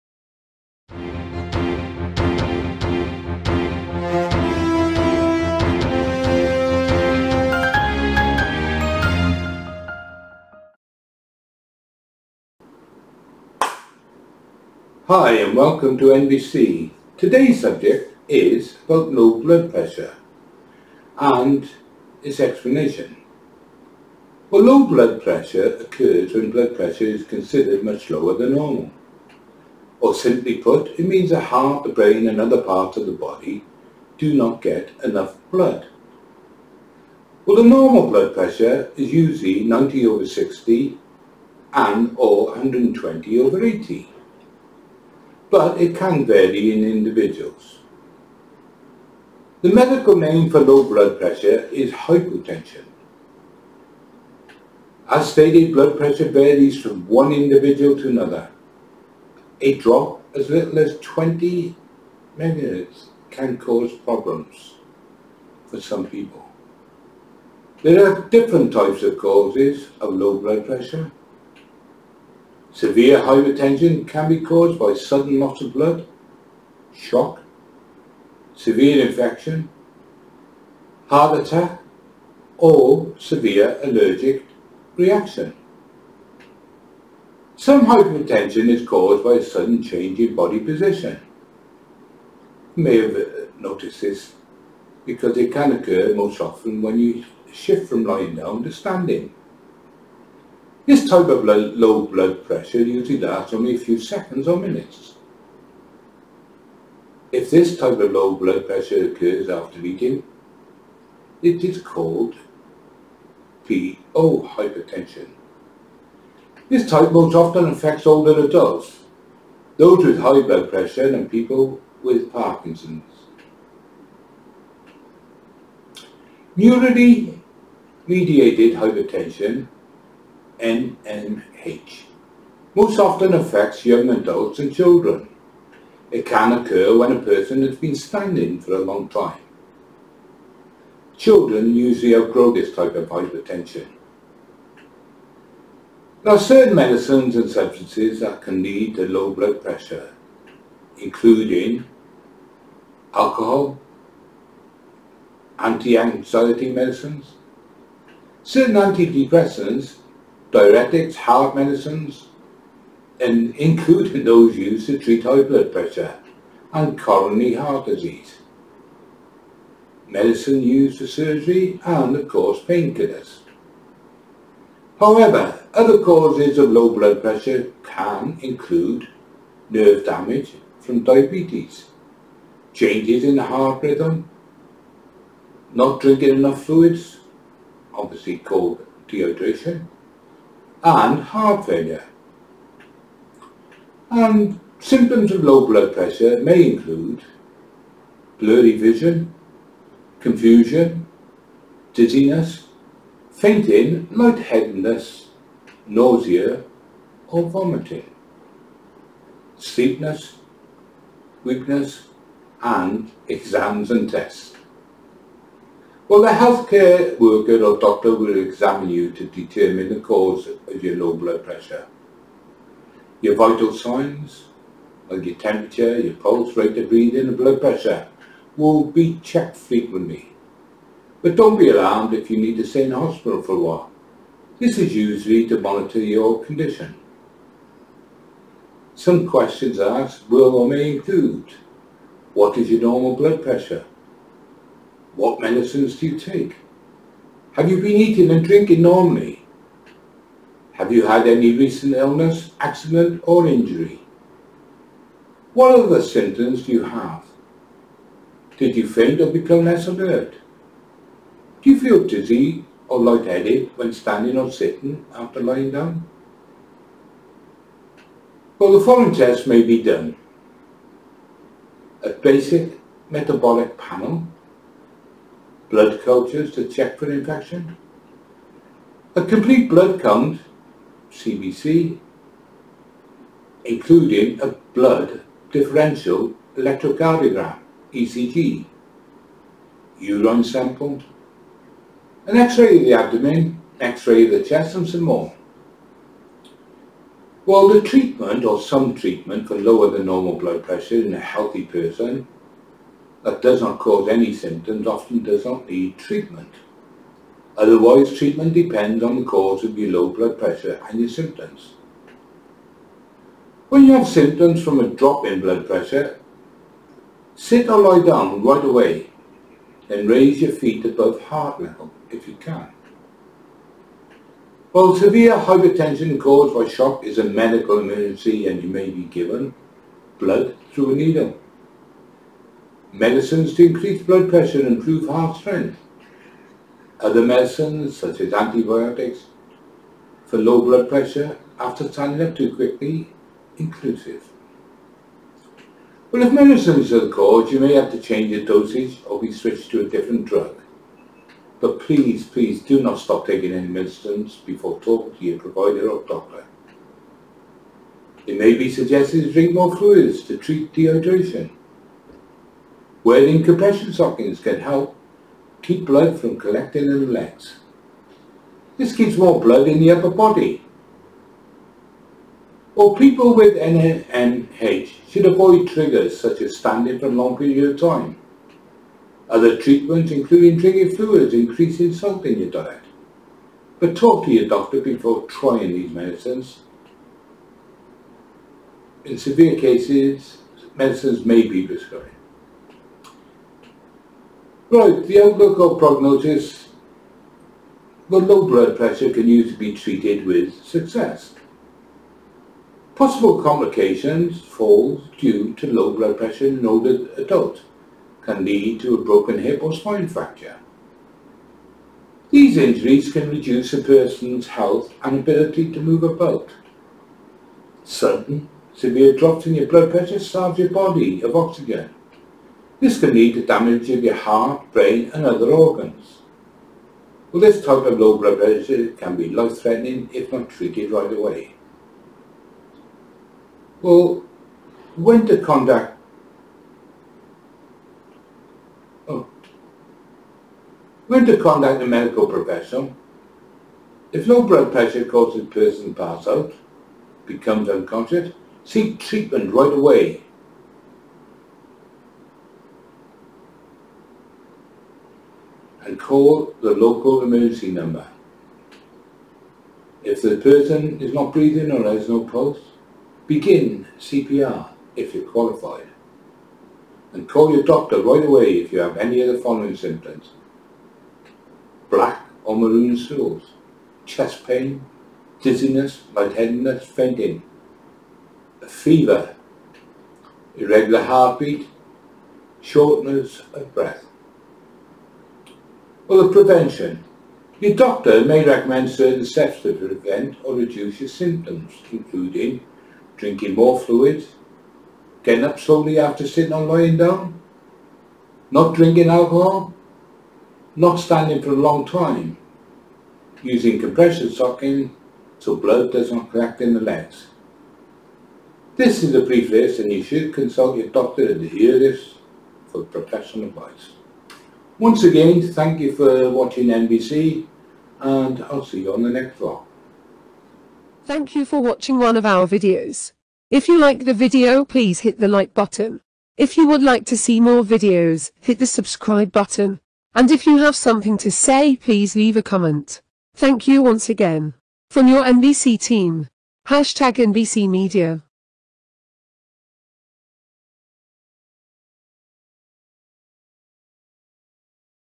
low blood pressure
Low blood pressure explained

Low blood pressure occurs when blood pressure is considered much lower than normal.
simply put means the heart, brain, and other parts of the body do not get enough blood. Normal blood pressure is usually 90/60 mmHg and 120/80 mmHg..
but can vary in individuals

The medical name for low blood pressure is hypotension.

Some Causes
As stated Blood pressure varies from one individual to another. A drop as little as 20 mmHg, can cause problems for some people. There are different types and causes of low blood pressure.

Severe hypotension can be caused by sudden loss of blood (shock), severe infection, heart attack, or severe allergic reaction (anaphylaxis).

some hypotension is caused by a sudden change in body position. This occurs most often when you shift from lying down to standing. This type of low blood pressure usually lasts only a few seconds or minutes.
If this type of low blood pressure occurs after eating, it is called  PO hypotension. This type most often affects older adults, those with high blood pressure, and people with Parkinson disease.

Neurally mediated hypotension (NMH) most often affects young adults and children. It can occur when a person has been standing for a long time. Children usually outgrow this type of hypotension.

Certain medicines and substances can lead to low blood pressure, including:

Alcohol
Anti-anxiety medicines
Certain antidepressants
Diuretics
Heart medicines, including those used to treat high blood pressure
and coronary heart disease
Medicines used for surgery
Painkillers

However Other causes of low blood pressure can include:

More details available on video

Translated titles:
Presión arterial baja

Niedriger Blutdruck

Pression artérielle basse

Pressão arterial baixa

निम्न रक्तचाप

انخفاض ضغط الدم

低血压#nbc

низкое кровяное давление

ความดันโลหิตต่ำ

Düşük tansiyon